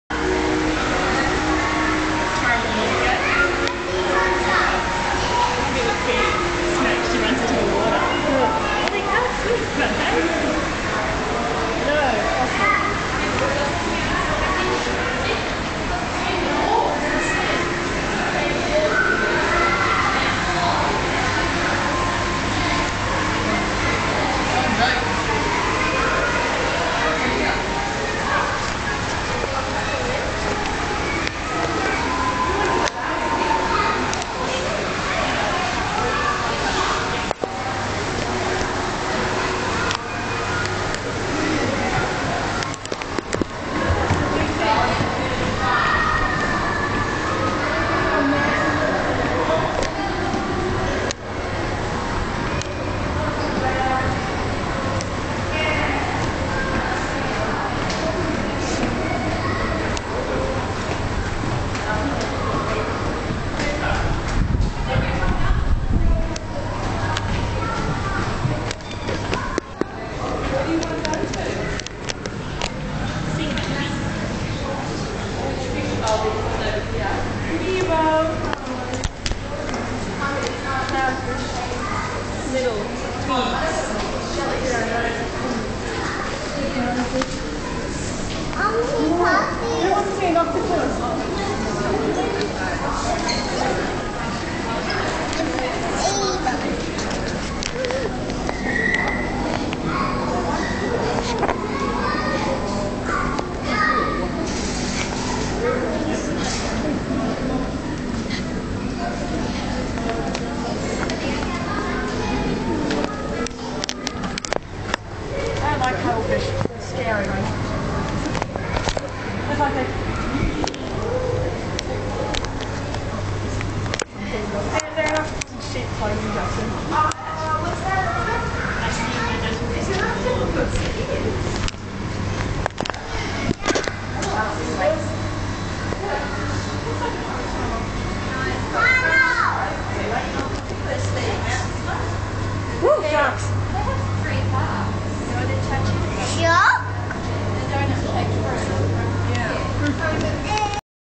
The Melbourne Aquarium 5
A group of my friends and I went to the Melbourne Aquarium This series of videos documents what we saw.